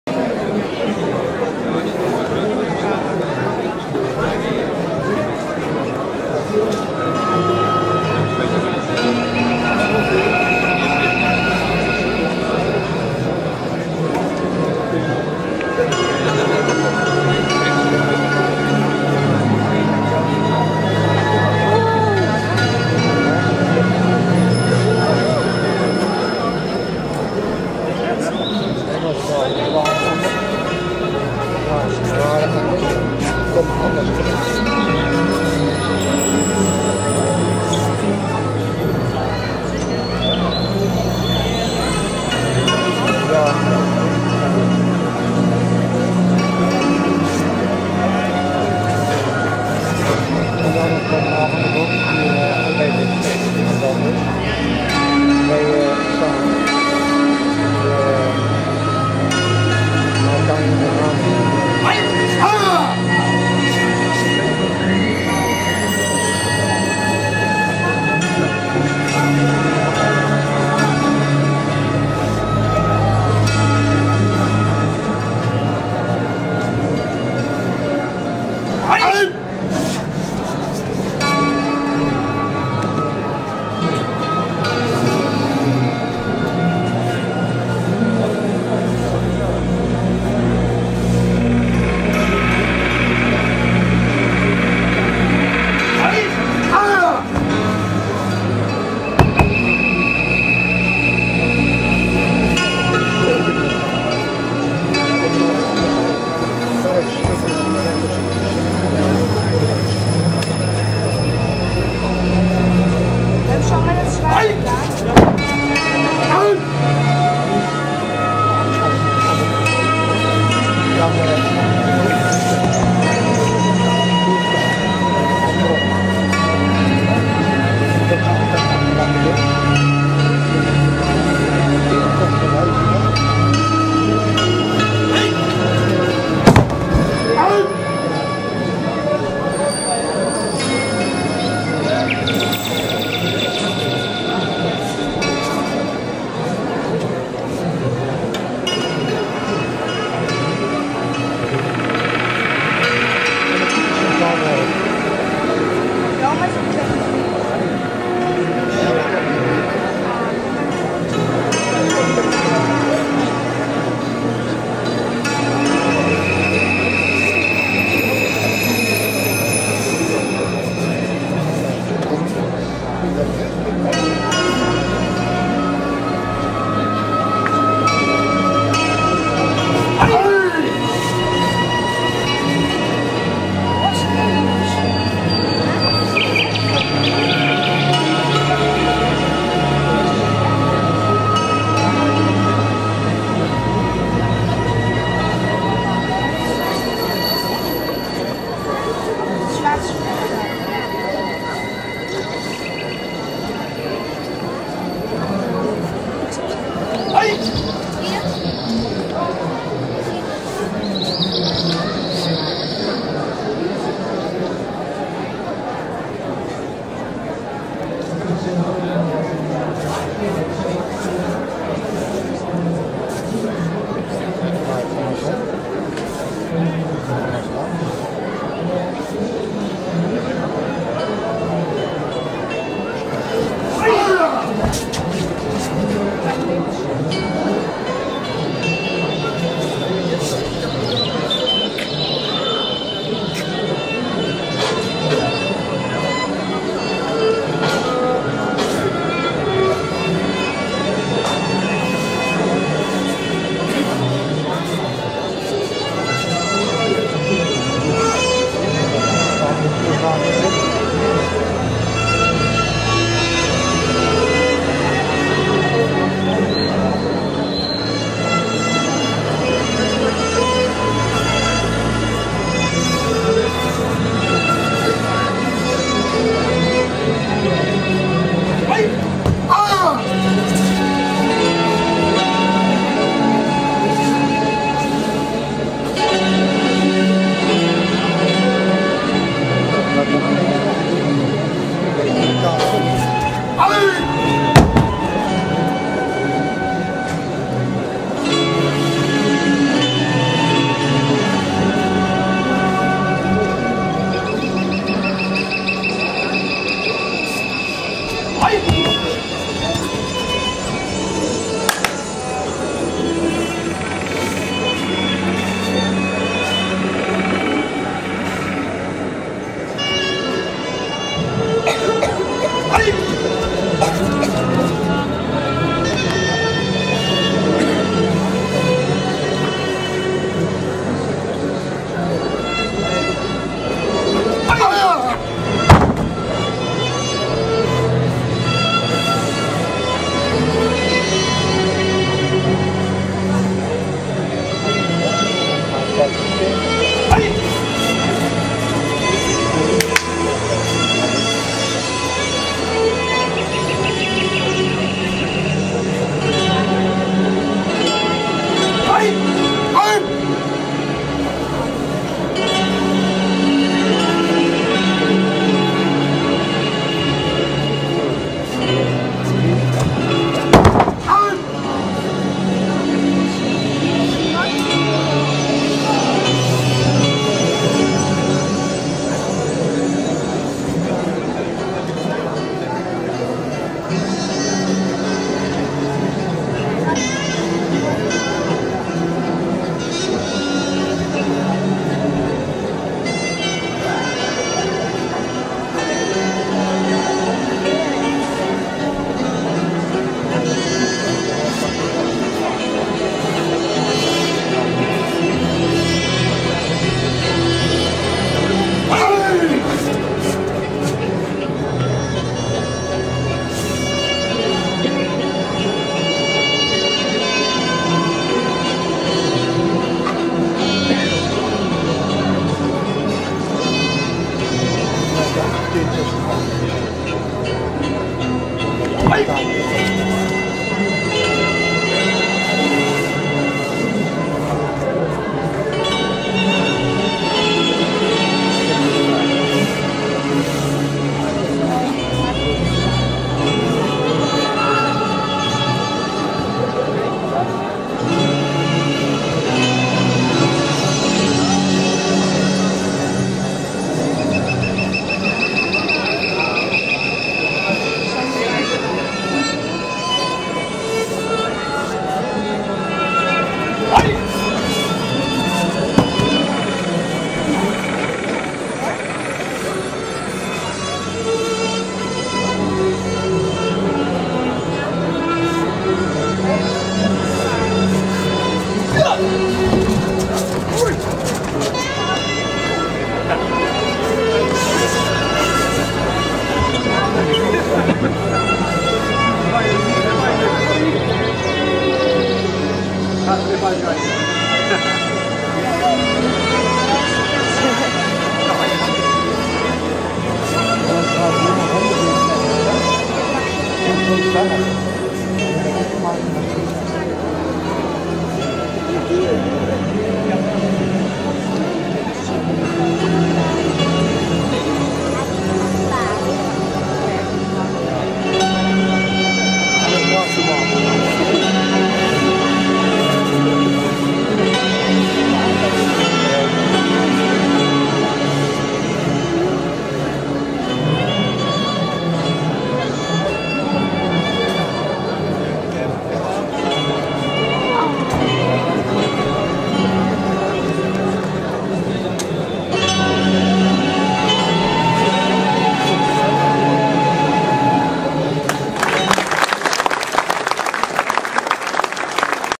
Judo, Kime no Kata (Busen) demonstratie WK judo 2009
Demonstration by Edgar Kruyning World Championships judo 2009
Ahoy Rotterdam